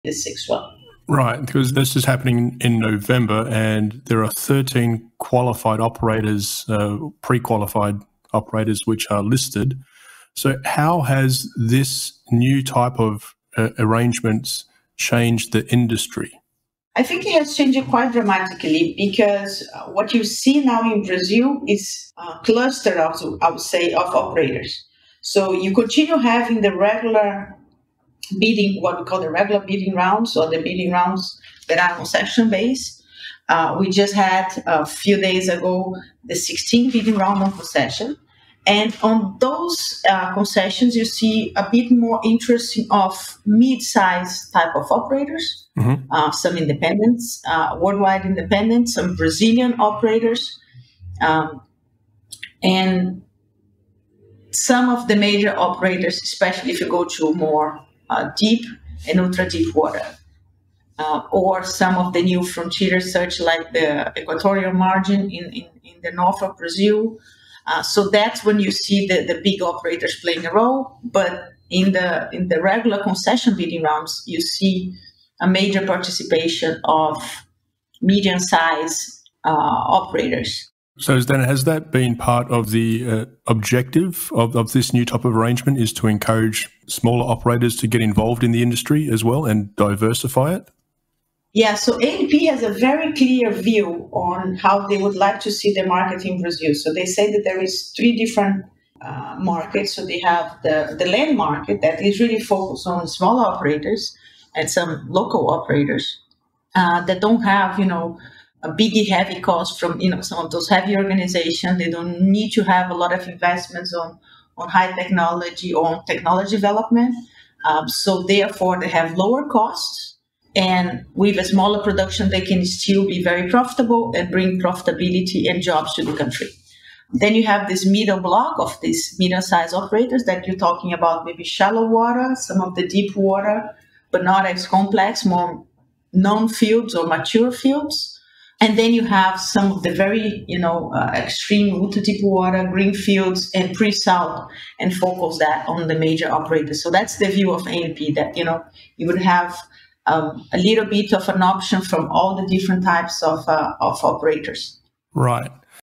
Brazil's ANP strategy encourages mid-sized and independent oil and gas operators, Ana Zambelli
Brazil's government regulator, the National Agency of Petroleum, Natural Gas and Biofuels, has a clear strategy based on three tiers, onshore, shallow water, and deep- and ultra-deep, exploration, according to Zambelli. This framework is designed to encourage the participation of mid-sized and independent operators in the Brazilian oil and gas industry.